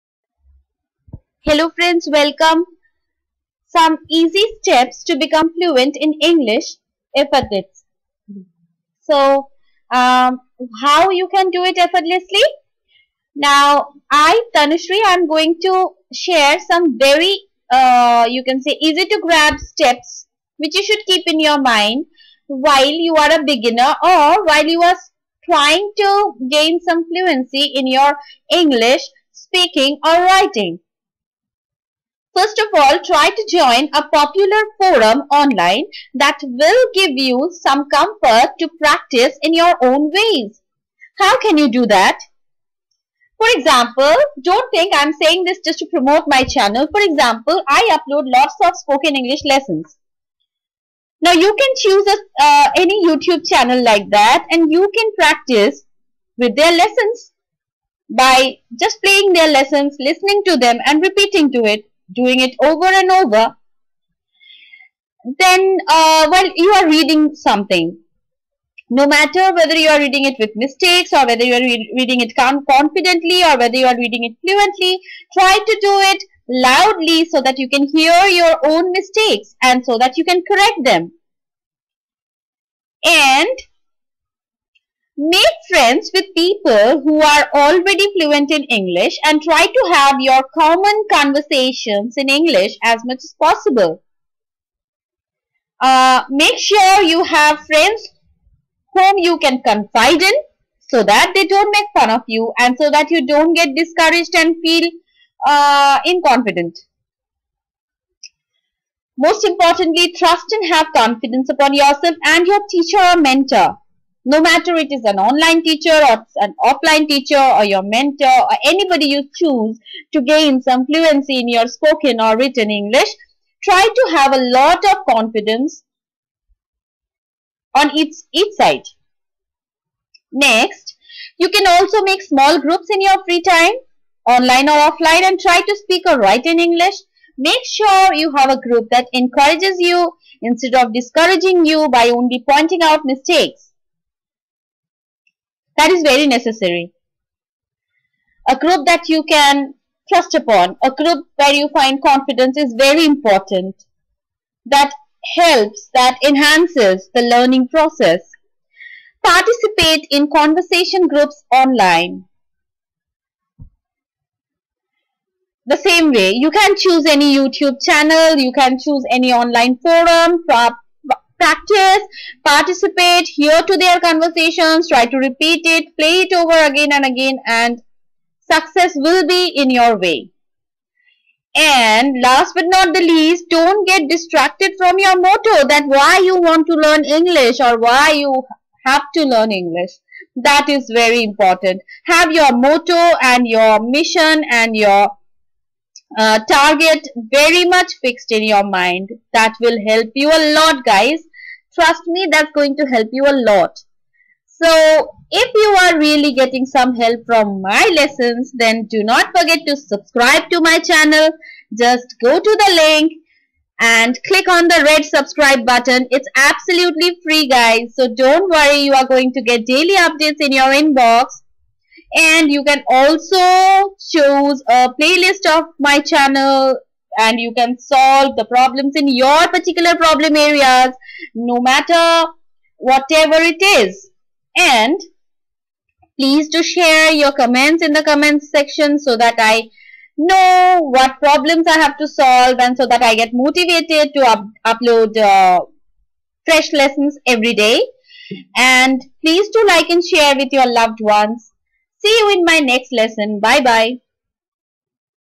Fluency in English without effort...
In this lesson I have shared some very easy and basic steps that will help you in gaining fluency...